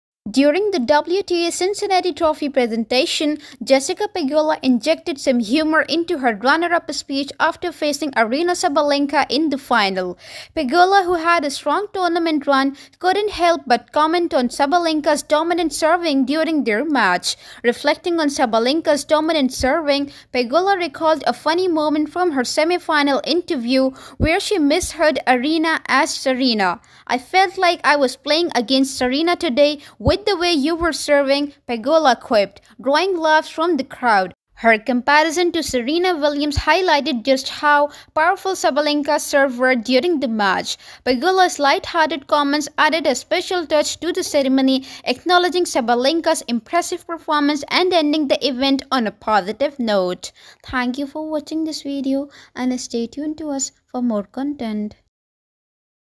Jessica Pegula leaves Aryna Sabalenka in Stitches with Serena Williams Comment in Speech 😂
During the WTA Cincinnati trophy presentation, Jessica Pegula injected some humor into her runner-up speech after facing Aryna Sabalenka in the final. Pegula, who had a strong tournament run, couldn’t help but comment on Sabalenka’s dominant serving during their match.

Reflecting on Sabalenka’s dominant serving, Pegula recalled a funny moment from her semifinal interview where she misheard "Aryna" as "Serena."

"It felt like I was playing against Serena today with the way you were serving," Pegula quipped, drawing laughs from the crowd. Her comparison to Serena Williams highlighted just how powerful Sabalenka's serves were during the match.

Pegula’s light-hearted comments added a special touch to the ceremony, acknowledging Sabalenka's impressive performance and ending the event on a positive note.